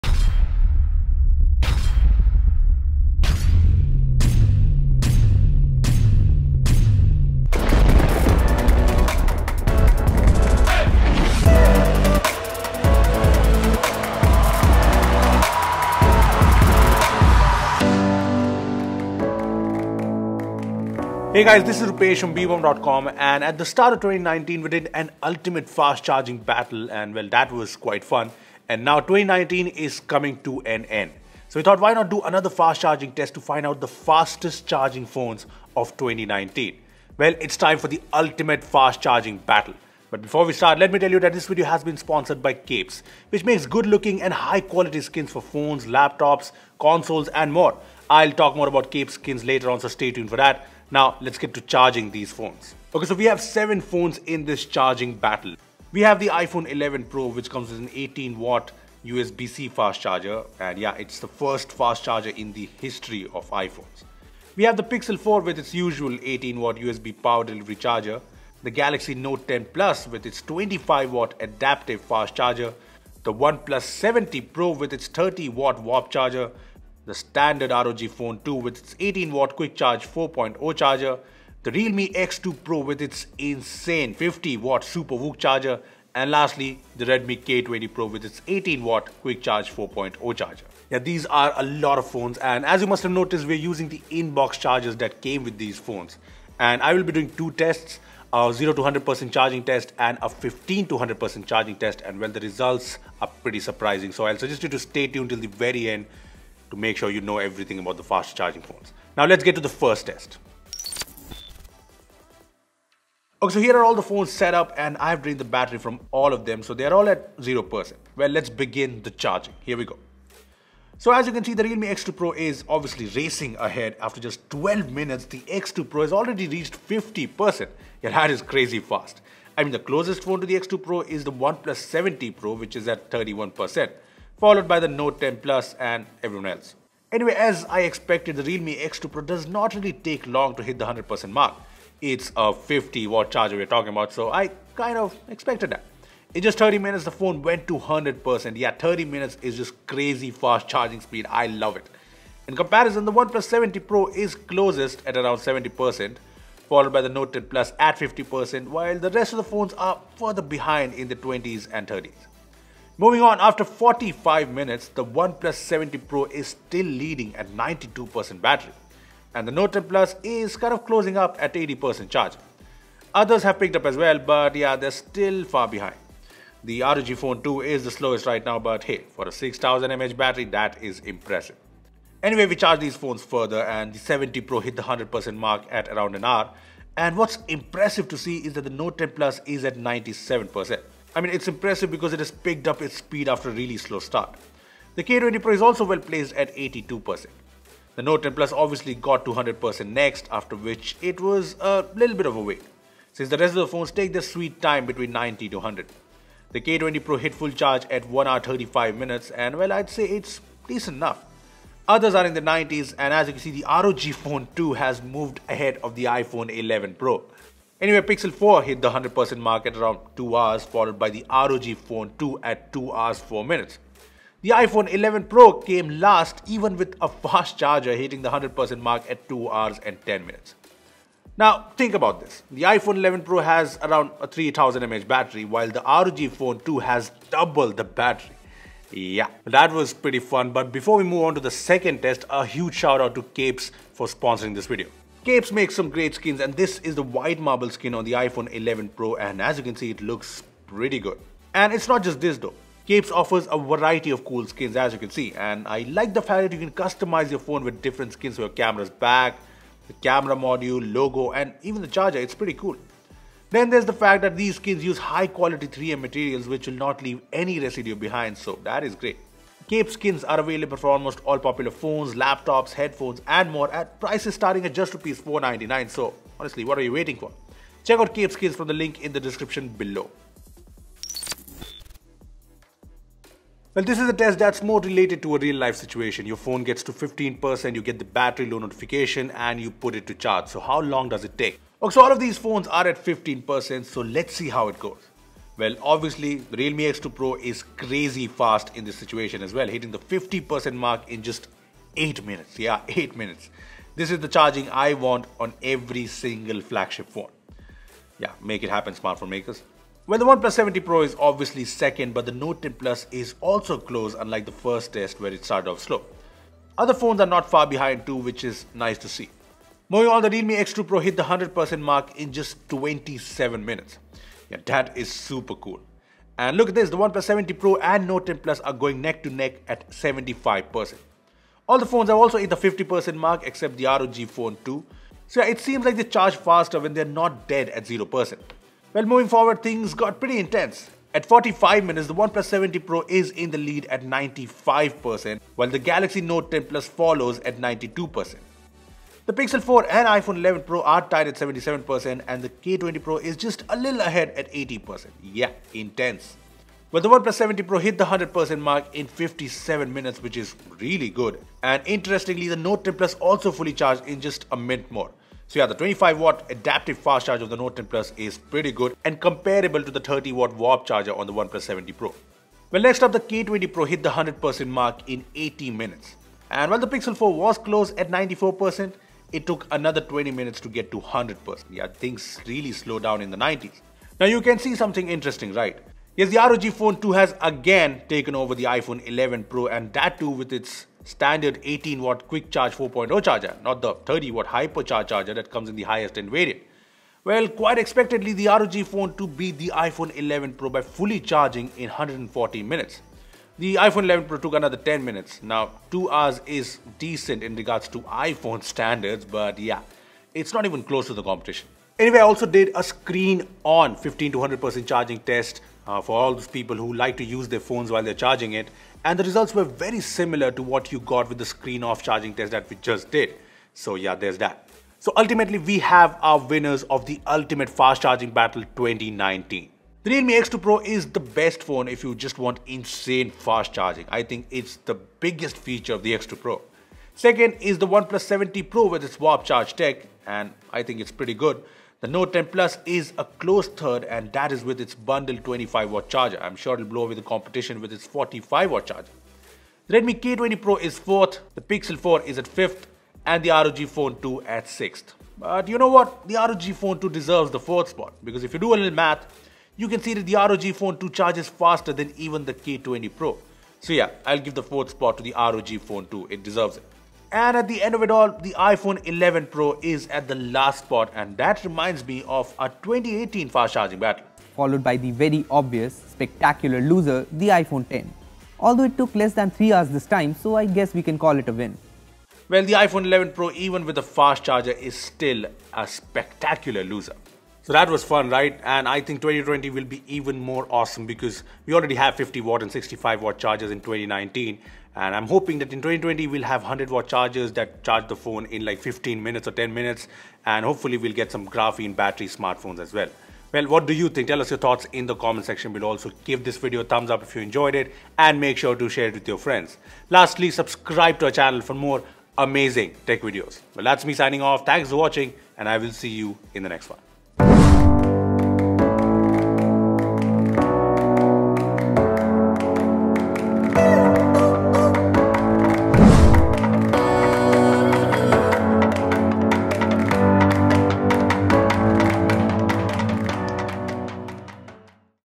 The Ultimate Fast Charging Battle - 2019
We did a fast charging test at the start of 2019, and now that 2019 is coming to an end, we thought why not do another fast charging test.

Well, we are pitting iPhone 11 Pro vs Pixel 4 vs Galaxy Note 10+ vs OnePlus 7T Pro vs ROG Phone 2 vs Realme X2 Pro vs Redmi K20 Pro with their in-box fast chargers.

1. iPhone 11 Pro (3046mAh) - 18W USB PD
2. Pixel 4 ( 2800mAh)- 18W USB PD
3. Galaxy Note 10+ (4,300mAh) - 25W USB PD
4. Redmi K20 Pro (4,000mAh) - 18W QuickCharge 4.0
5. Realme X2 Pro (4,000mAh) - 50W Super VOOC
6. OnePlus 7T Pro (4,085mAh) - 30T Warp Charge
7. ROG Phone 2 (6,000mAh) - 18W Quick Charge 4.0